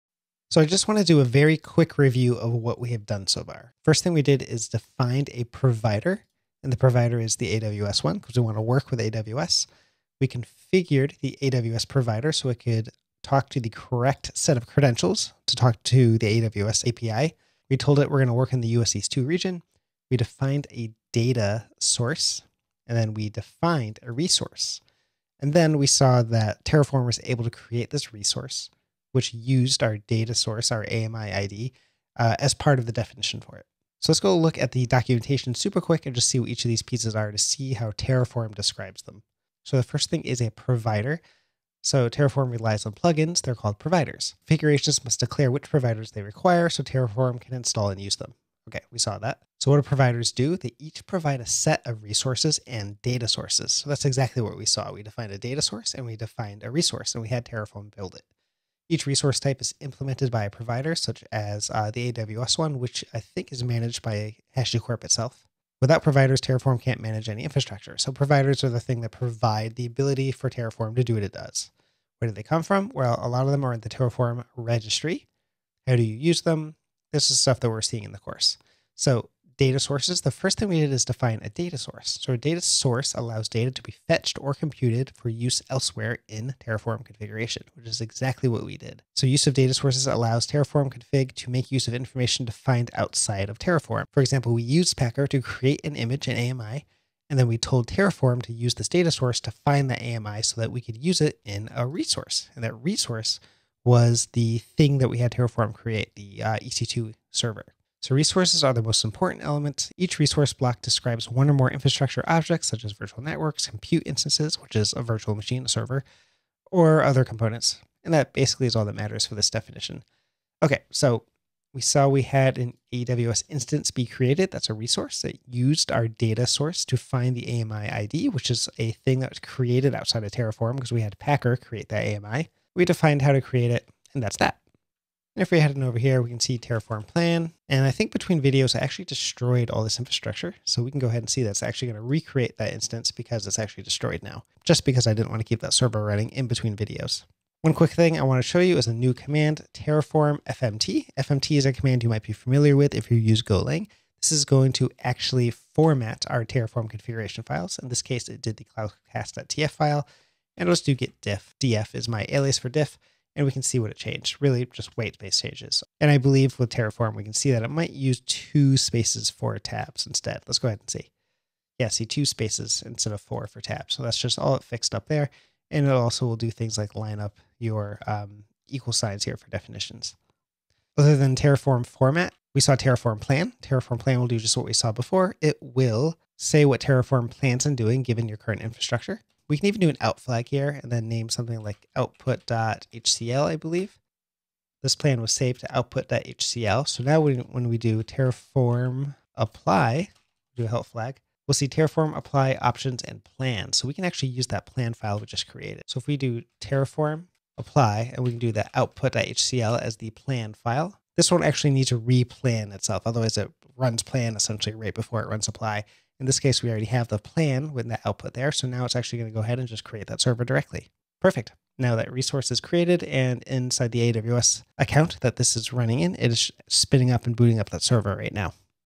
Review: Terraform Resources, Data Sources, and some Terraform helper commands.
We quickly review Terraform resources, data sources, format our Terraform configuration with the "terraform fmt" command, and show how we can use the "plan" command to generate a plan to apply.